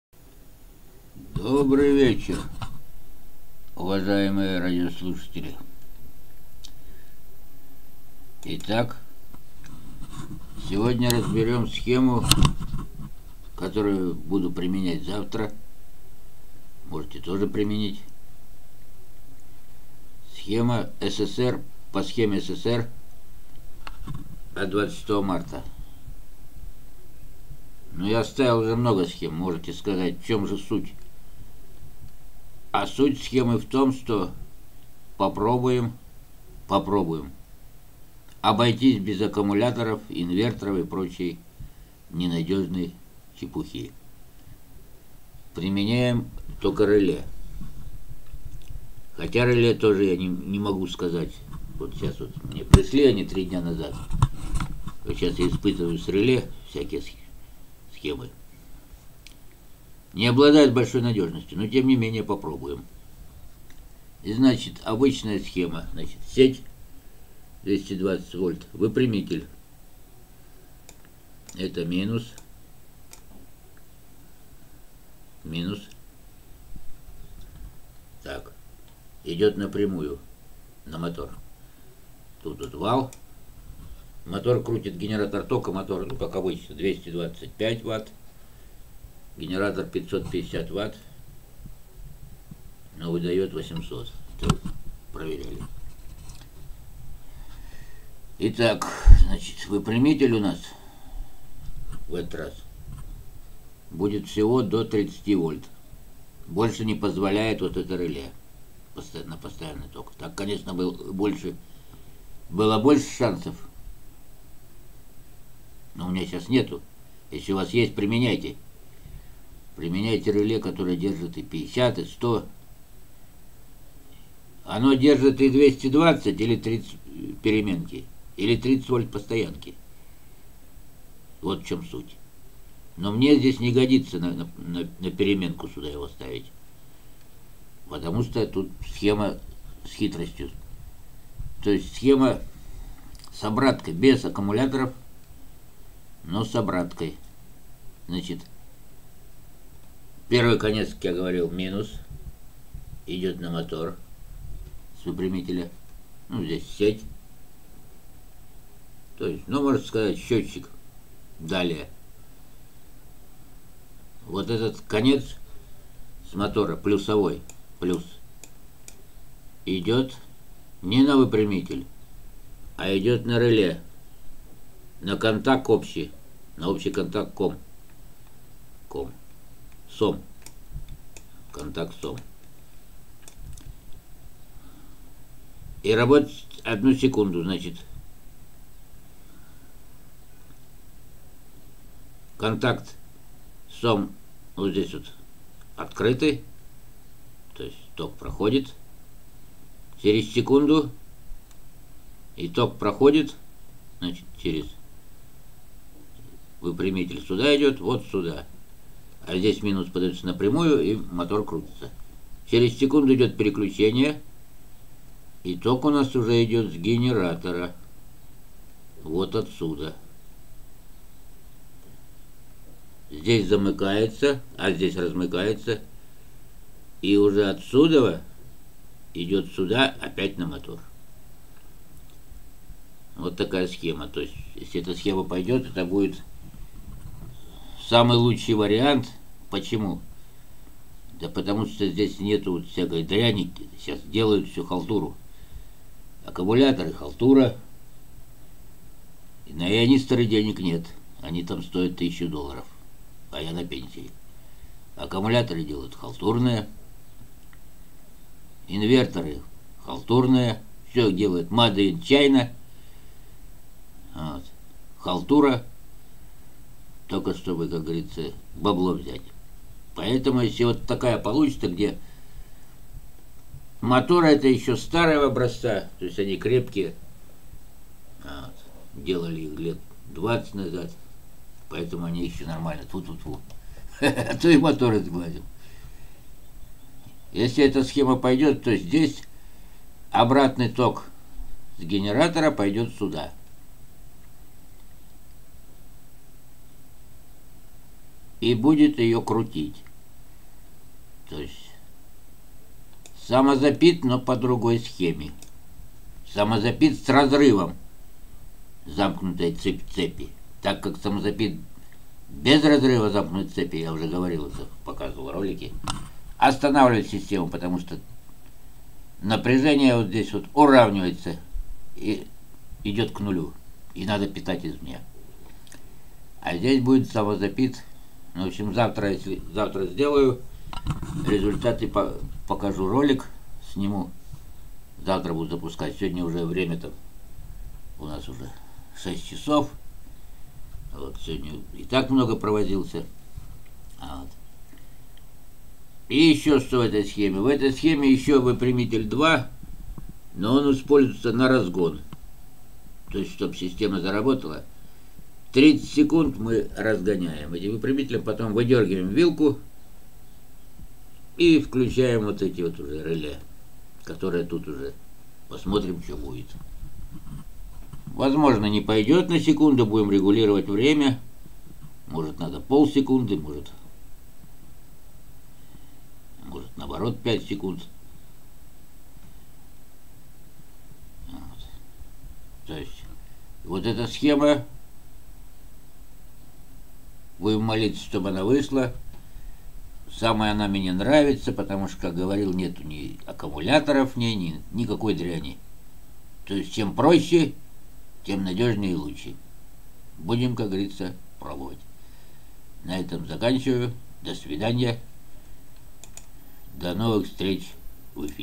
Бестопливный генератор СССР
Схема без акумуляторов и инверторов простая завтра ждите ролик где будут показаны испытания надемся что система заработает это будет самый лучший вариант . Так как не нужно ни акумуляторов ни инверторов которые по качеству и цене оставляют желать лучшего .